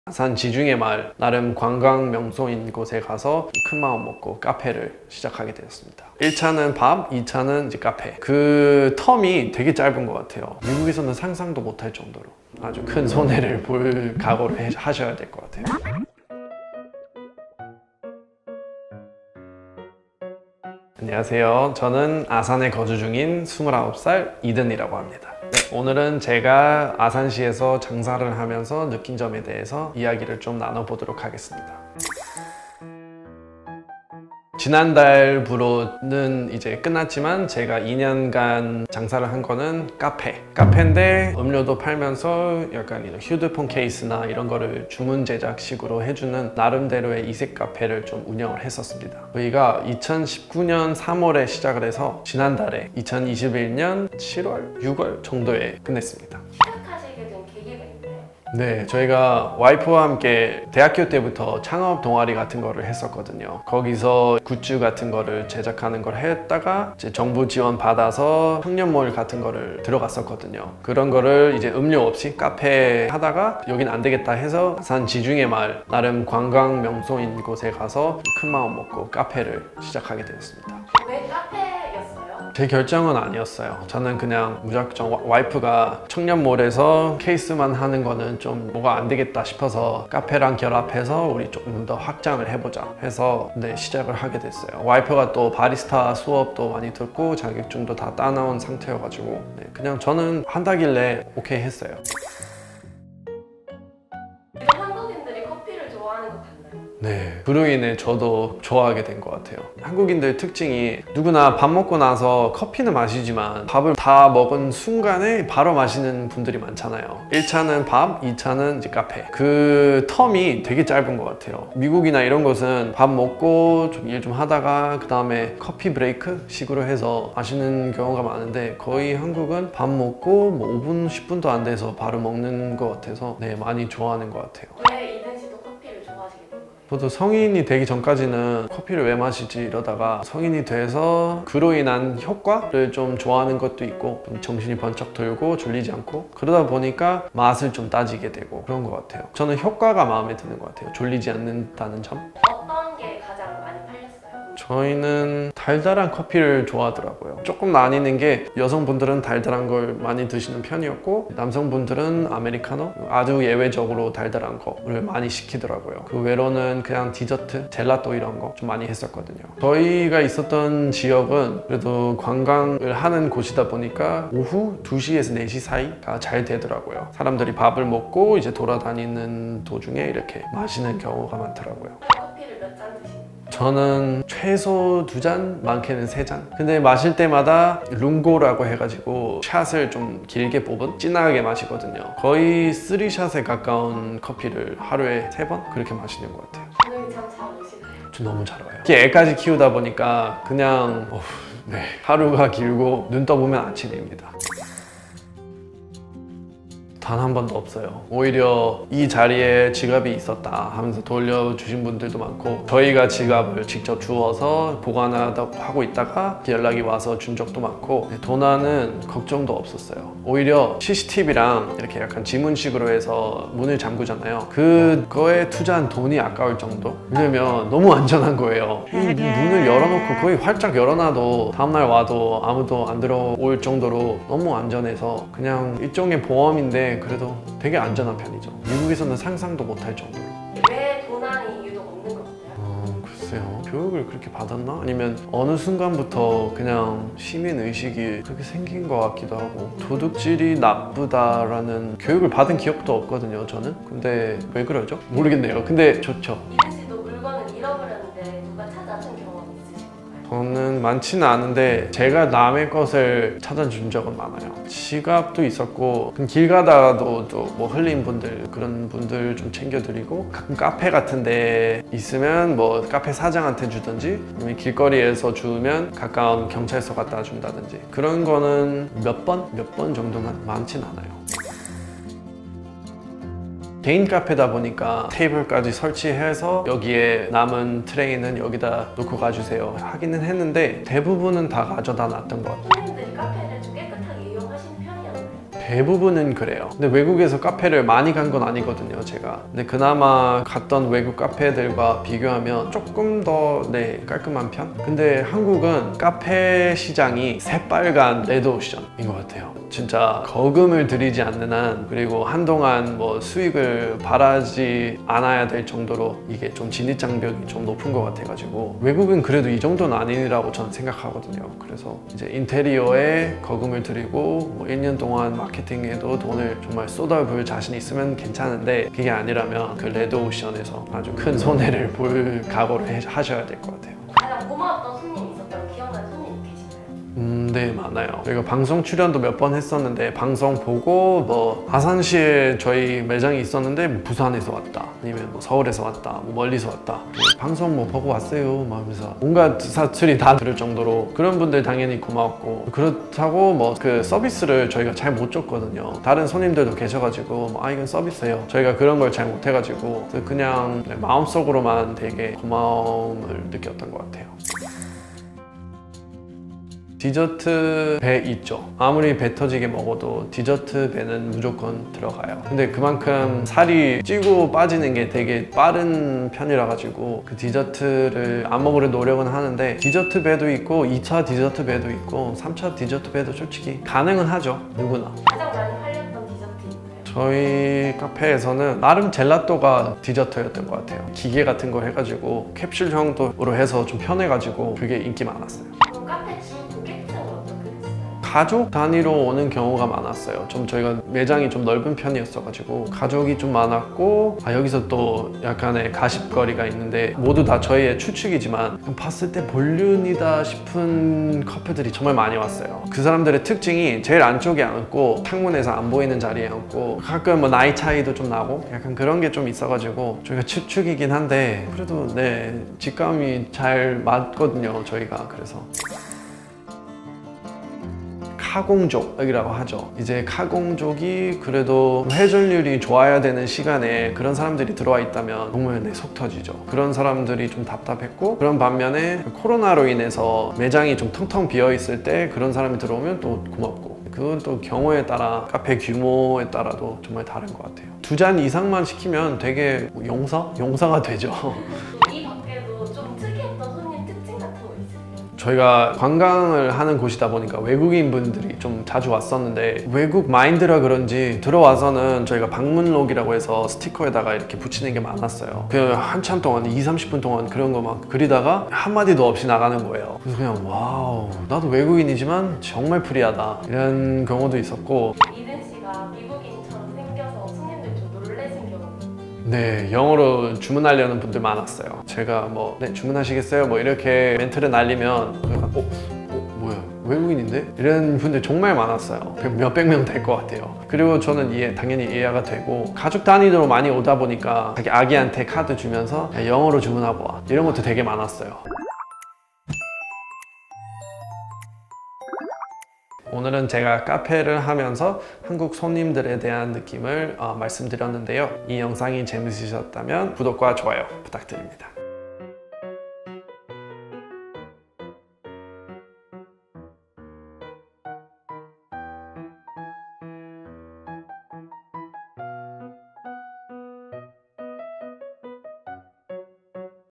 미국인이 한국에서 장사하면서 직접 목격한 한국 손님들 행동에 충격받은 이유
안녕하세요!

오늘은 미국인 이든 씨가

한국 카페에서 장사하면서 느꼈던 것들에 대해 말씀해주셨는데요!

지금 바로 같이 보러가시죠!!! :D